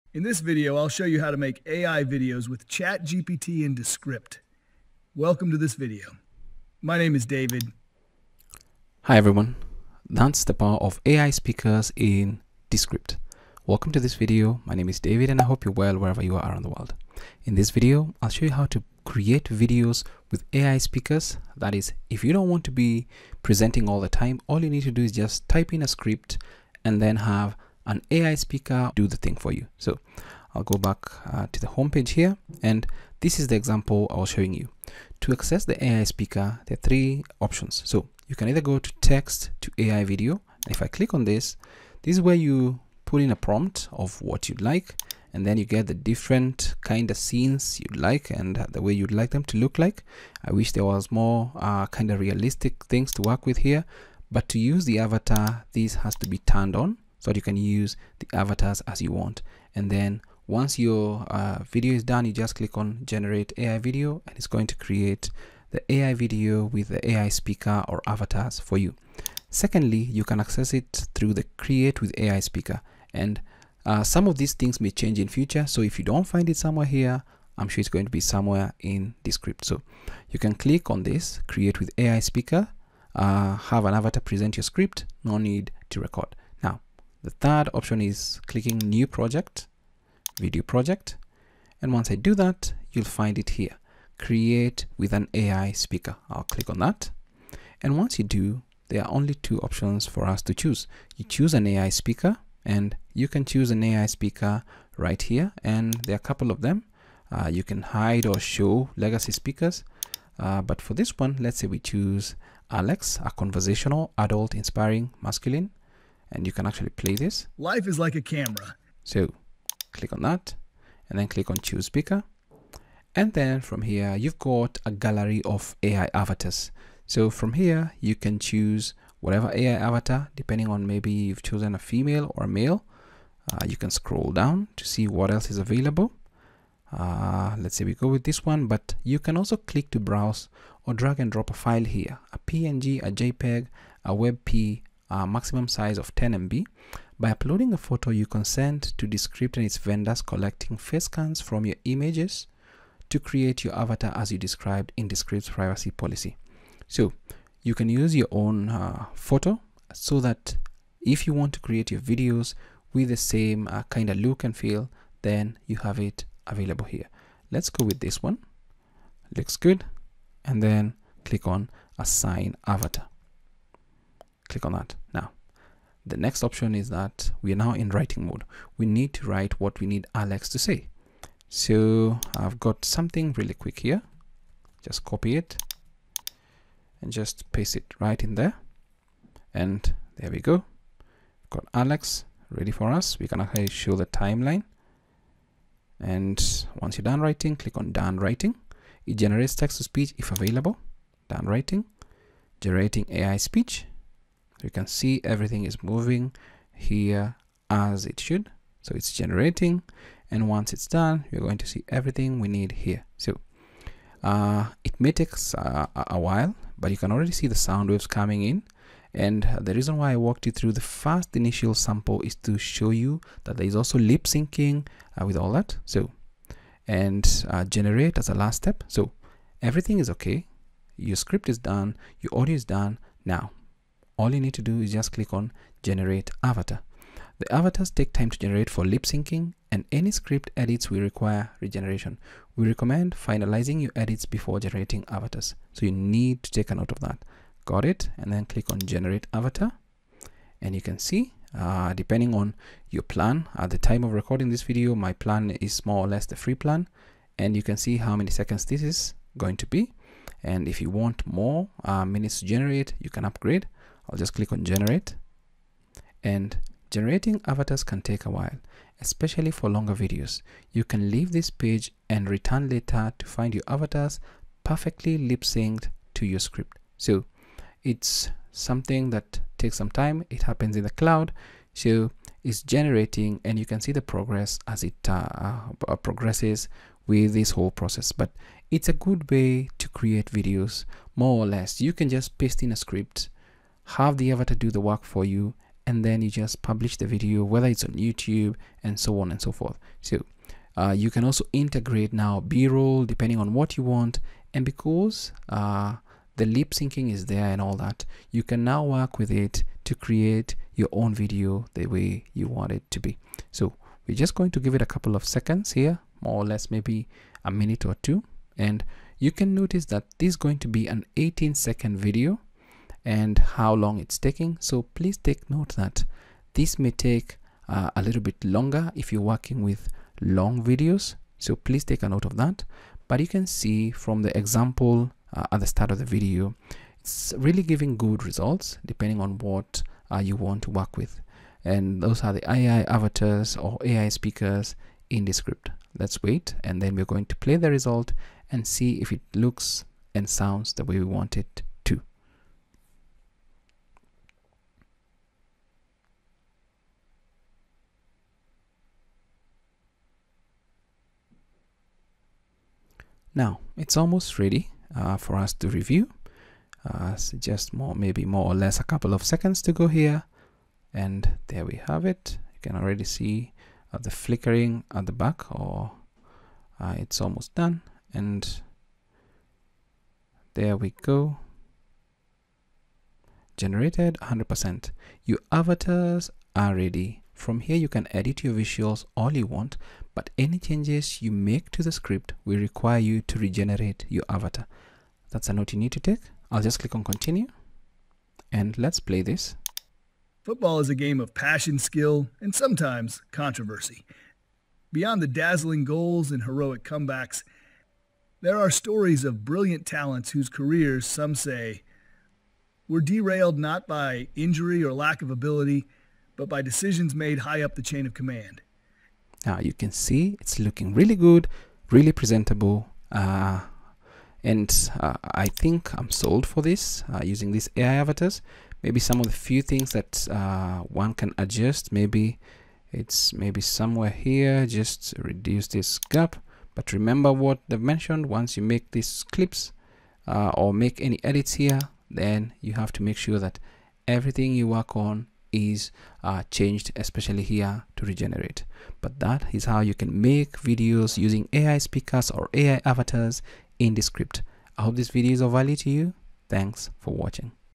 How to Make Video with AI Speakers or Avatars with Lip Syncing in Descript
In this tutorial, I'll show you How to Make Video with AI Speakers or Avatars with Lip-Syncing in Descript.

I hope this video has been of value.